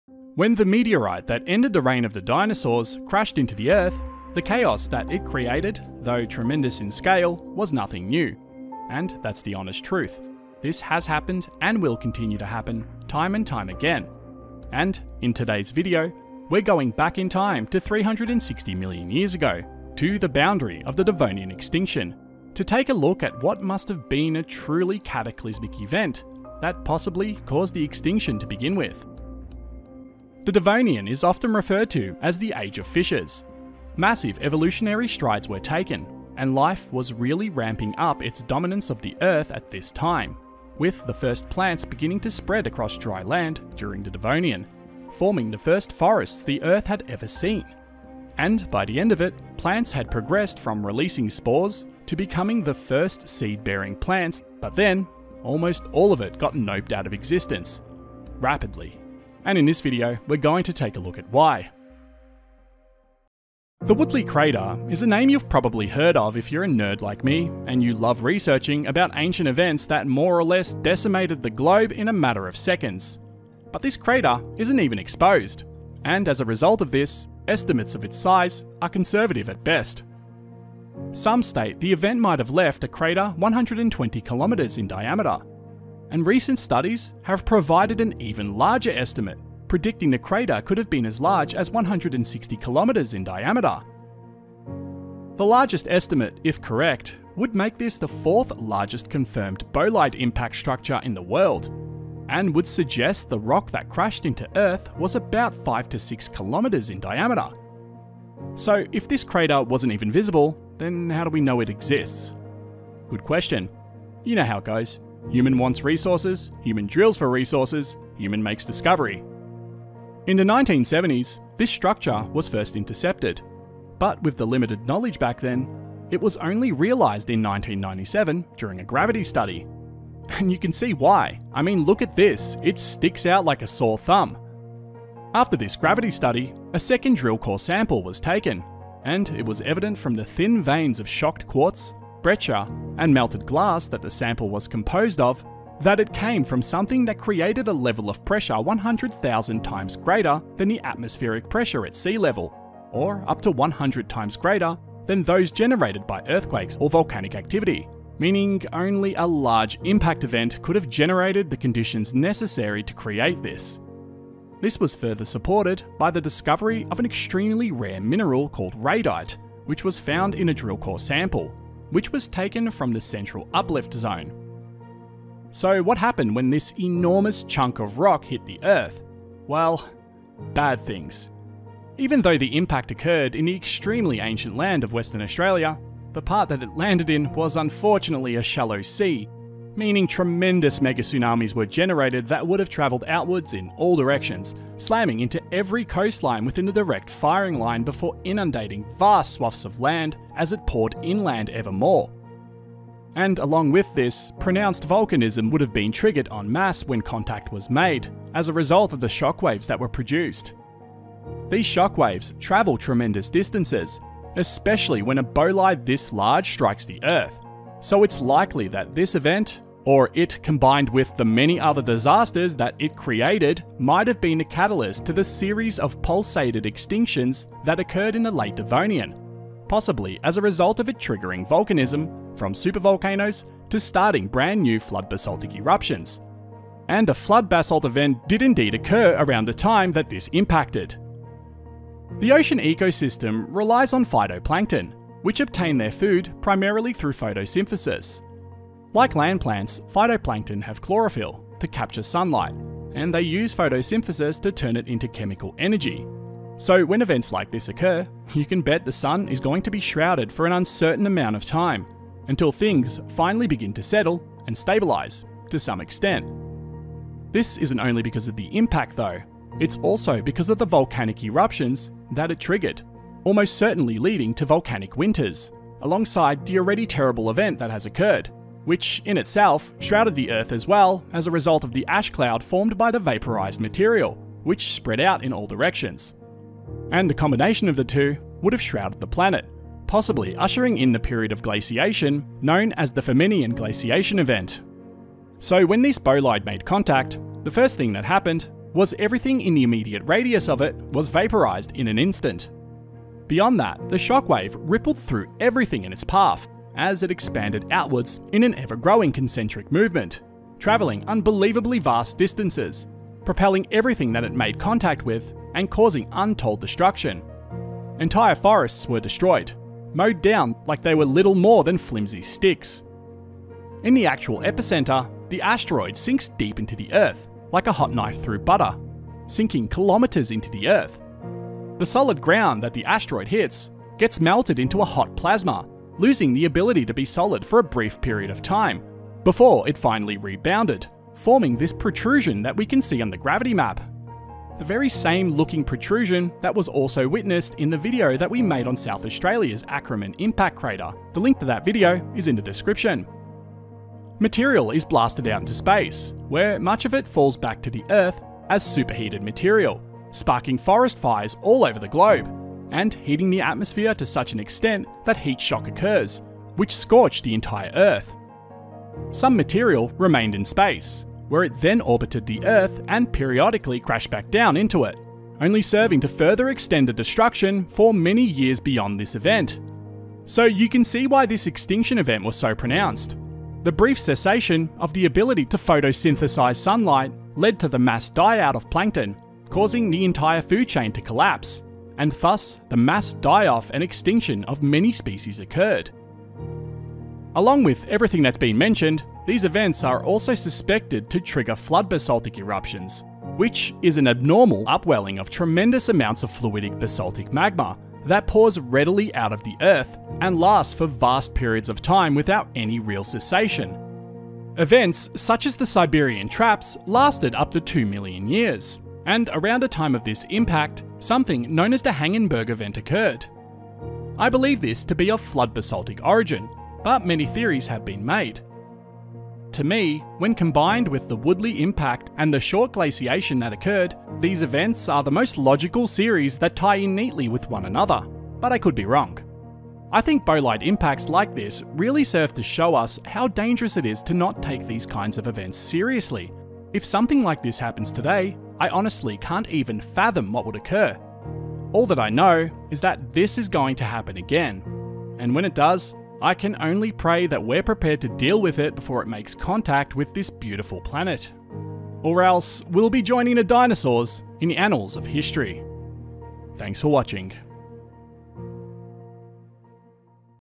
The Asteroid That Smashed Western Australia
The Woodleigh Crater is a tremendously large impact crater that's linked to the Devonian extinction crisis. In this video, we head back 360 millions years in time, and analyze one of the worst extinction events to have occurred, and what the Woodleigh impact did to possibly set off the chain of events that would eventually lead to this mass die off of flora and fauna.